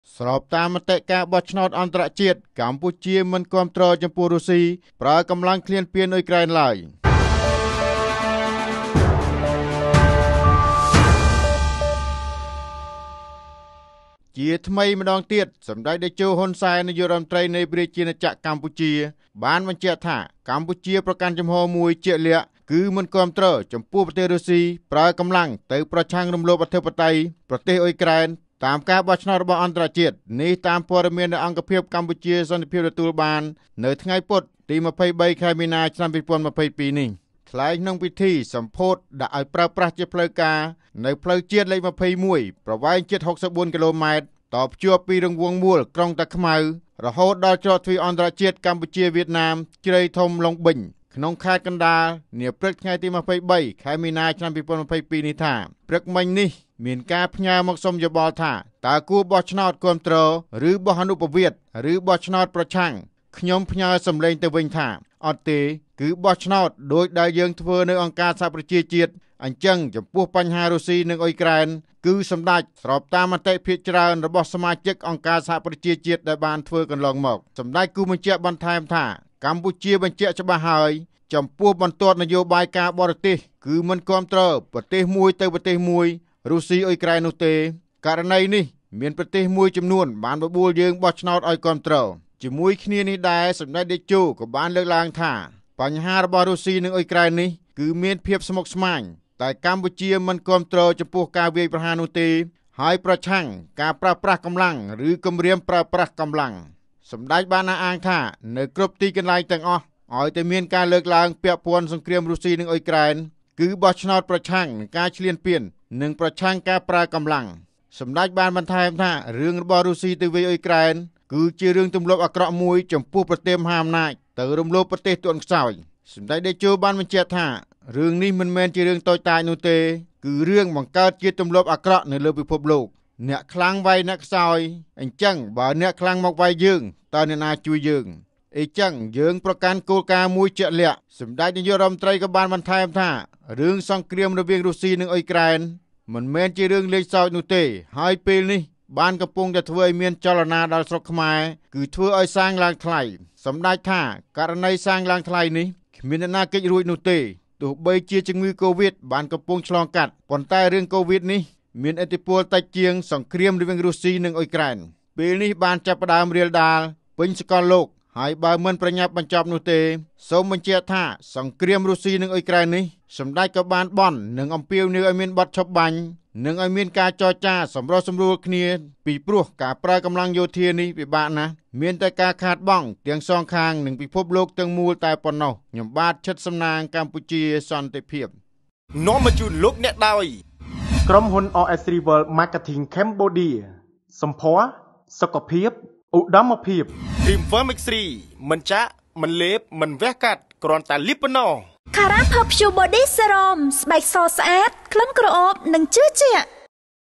ស្របតាមមតិការបោះ ឆ្នោត អន្តរជាតិ កម្ពុជា មិន គាំទ្រ ចំពោះ រុ ស្ស៊ី ប្រើ កម្លាំង ឈ្លានពានអ៊ុយក្រែនឡើយ
ស្រប តា មមតិកា របោះ ឆ្នោត អន្តរជាតិ កម្ពុជា មិន គាំទ្រ ចំពោះ រុ ស្ស៊ី ប្រើ កម្លាំង ឈ្លានពាន អ៊ុយ ក្រែ នឡើយ